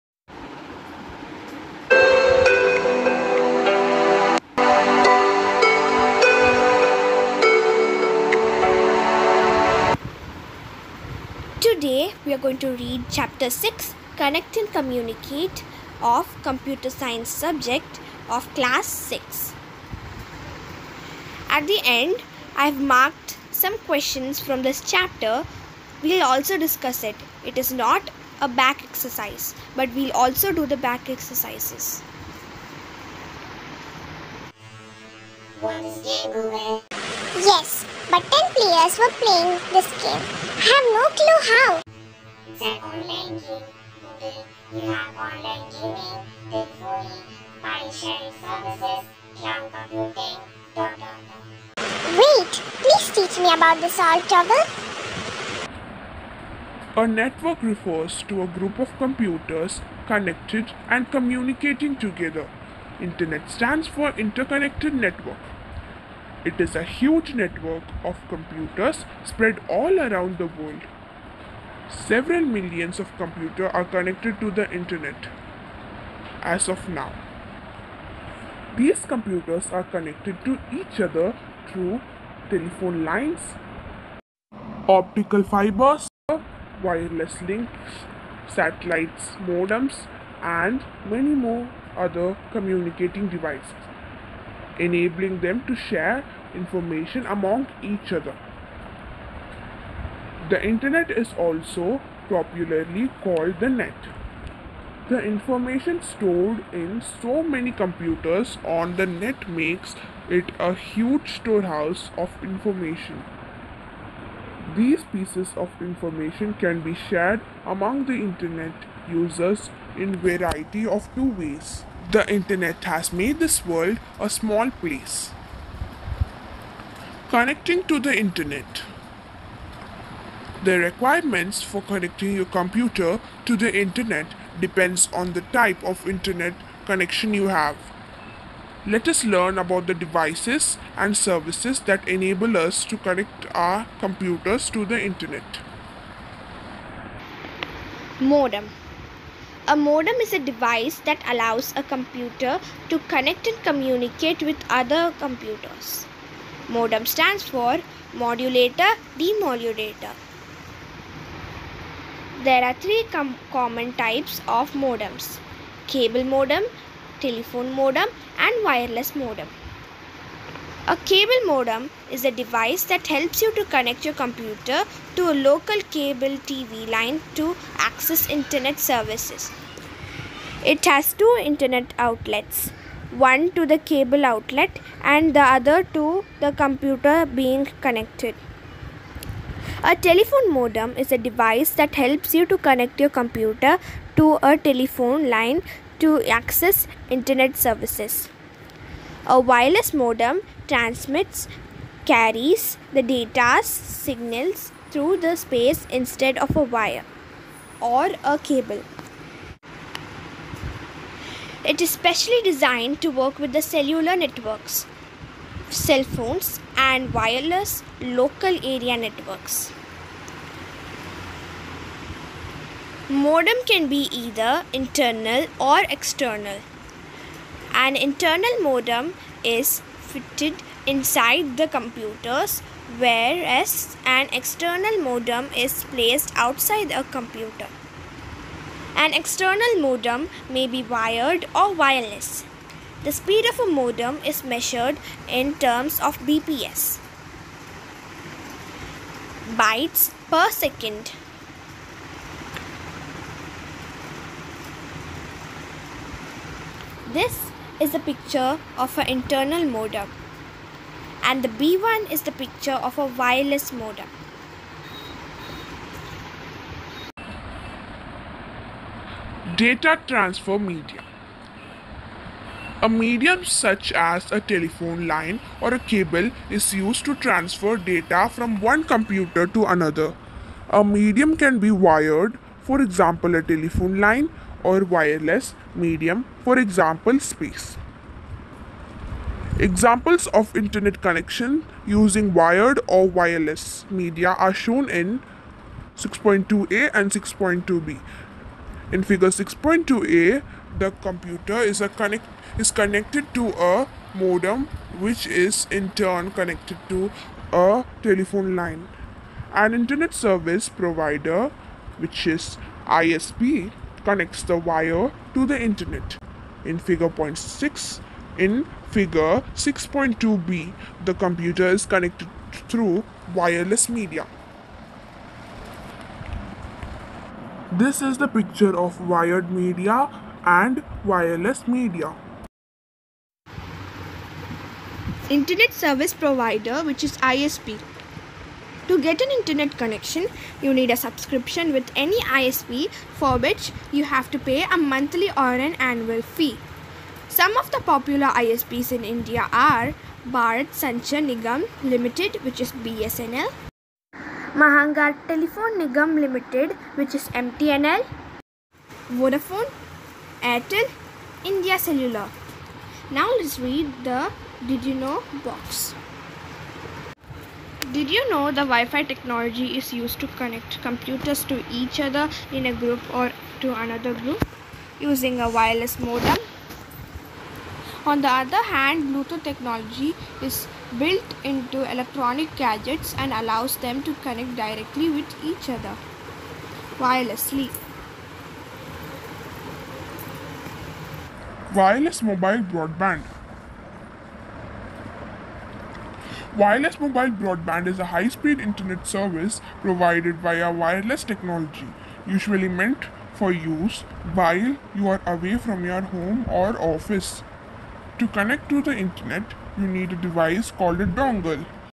Sky Channel | Computer chapter | Class 6 | Connect And Communicate | Part 1
Sky Channel Presents :
Class 6 chapter of computer Connect And Communicate

Comment on which chapter you want to be read by us and on how the video was

Check on some other recipie videos

bye👋🏻👋🏻